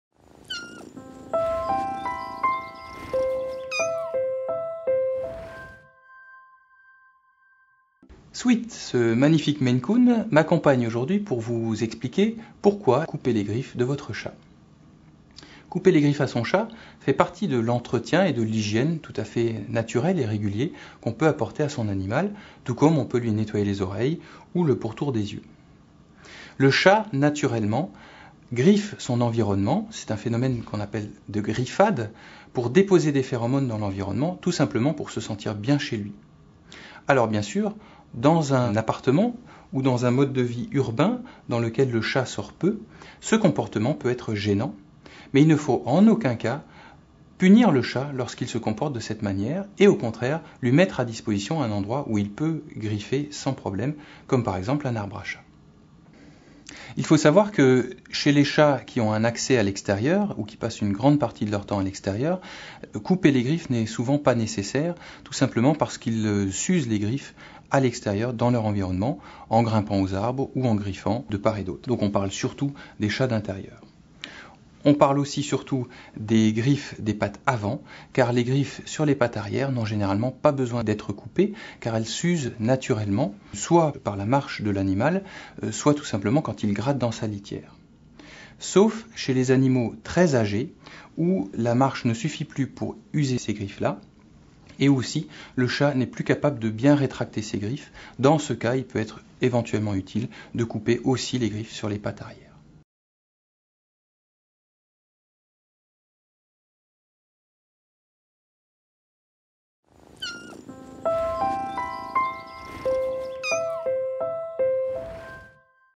Pourquoi et comment couper les griffes de mon chat ?⎜ROYAL CANIN®
Notre expert vétérinaire vous explique pouvoir couper les griffes de votre chat. Vous comprendrez pourquoi le chat a besoin de griffer, et comment prévenir de ce besoin.